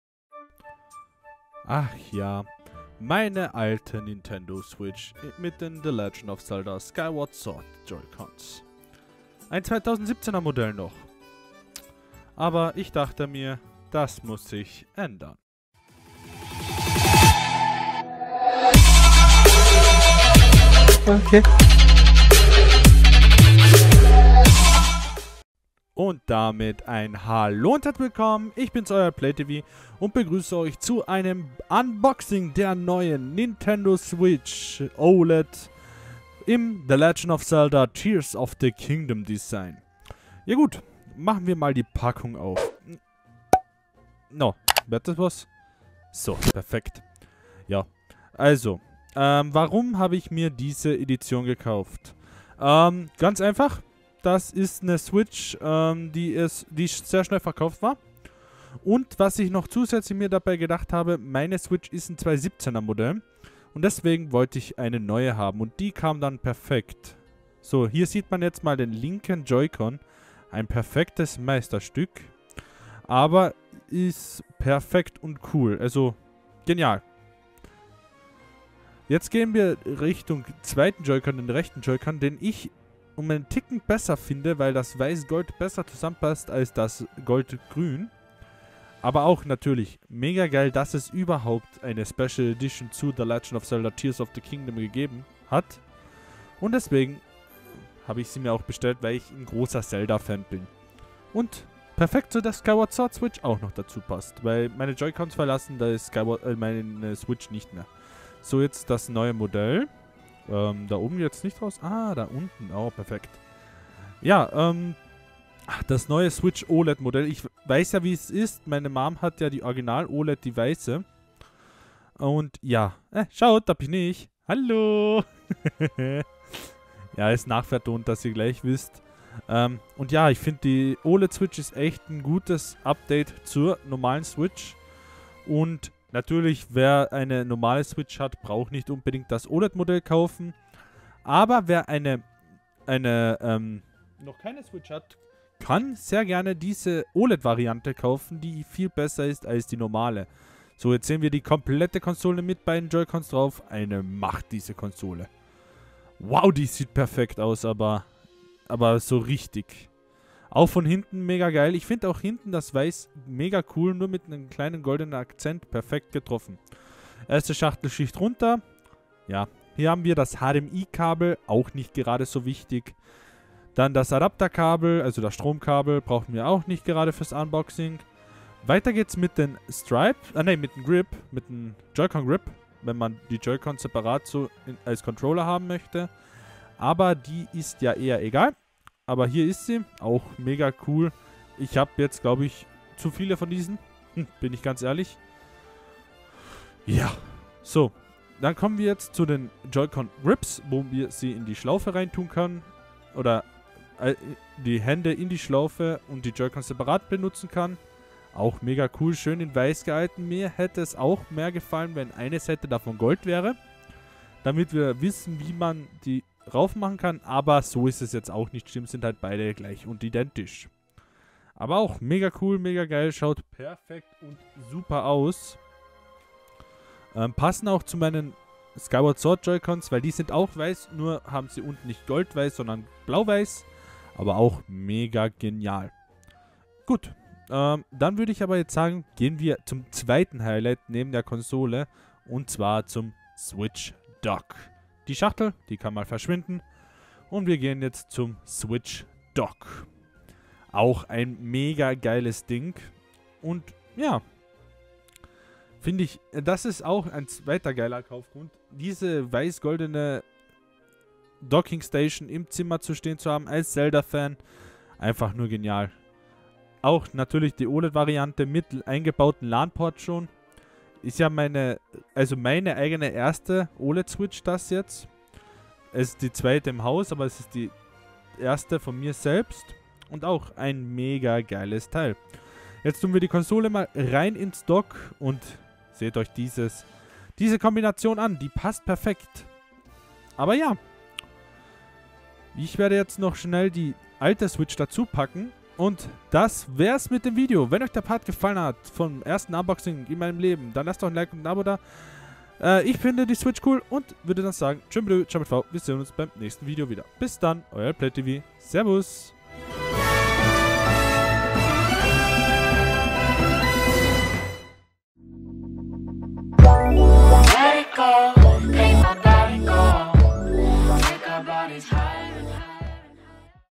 UNBOXING Nintendo Switch OLED The Legend of Zelda Tears of the Kingdom Edition | LPlayTV
Wenn es euch gefallen hat dann lasst doch eine Bewertung da und ein Abo
LG:. L Play

Viel Spaß